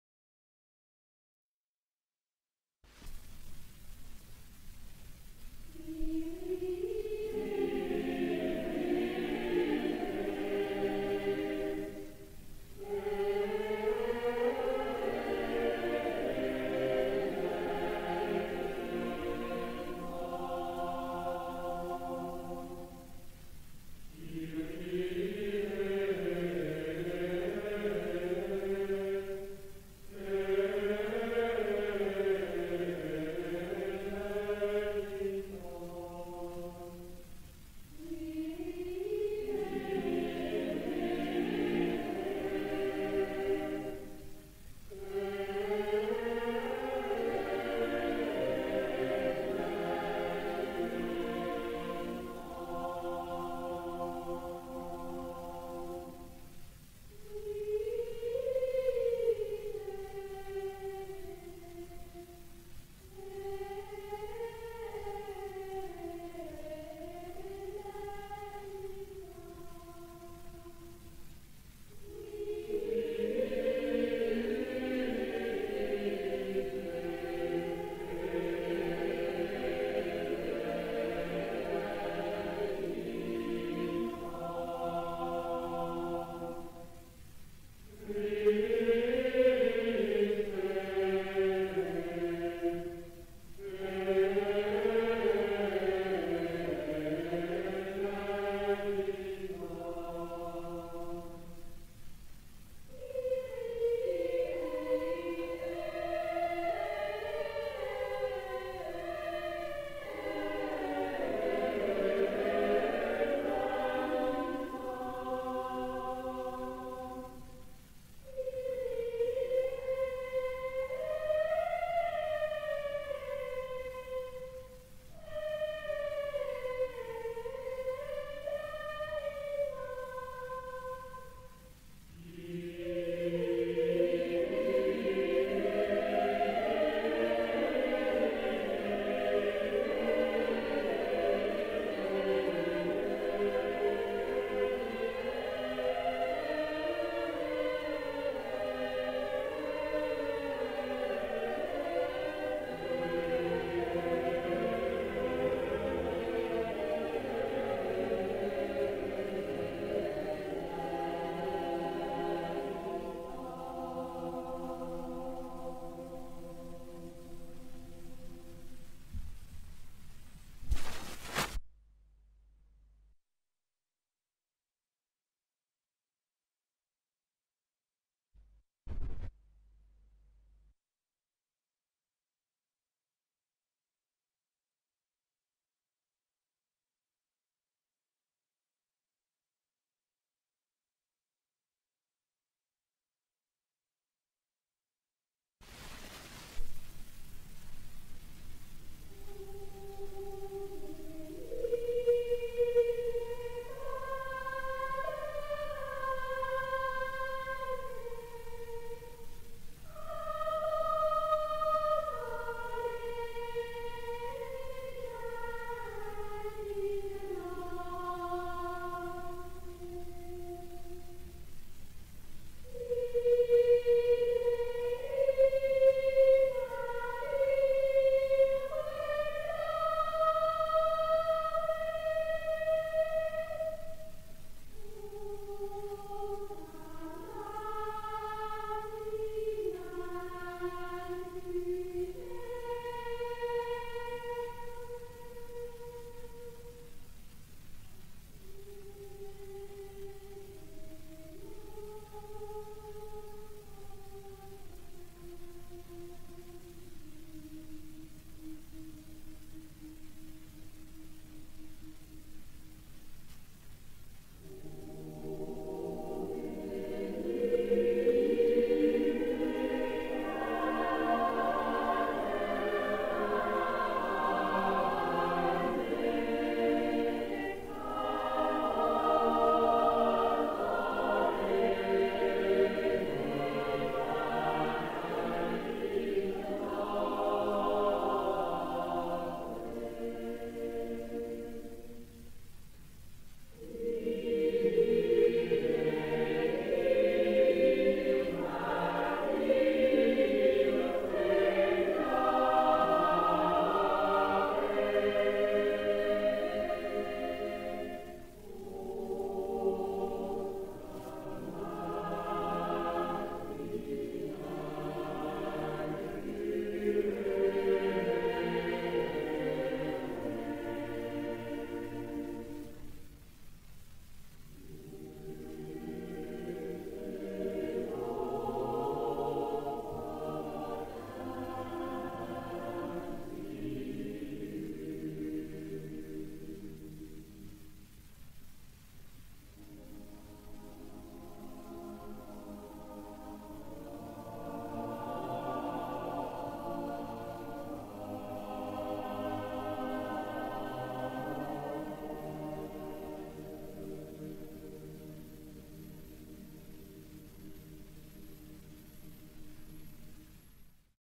Chant Grégorien de la Maîtrise de la Cathédrale de Dijon (Harmonisé par J. Samson)
Support d'enregistrement: Disque
Type de Disque: 78 tours (Shellac)
DAte de l'enregistrement: Années 1950 (?)

/!\ CE DISQUE PRÉSENTE DU CHANT GRÉGORIEN  EN ALTERNANCE AVEC DE LA POLYPHONIE COMPOSÉE PAR J. SAMSON. /!\

Pas de lien Musicologie Médiévale à propos de ce disque car il n'a pas été mis en ligne sur le site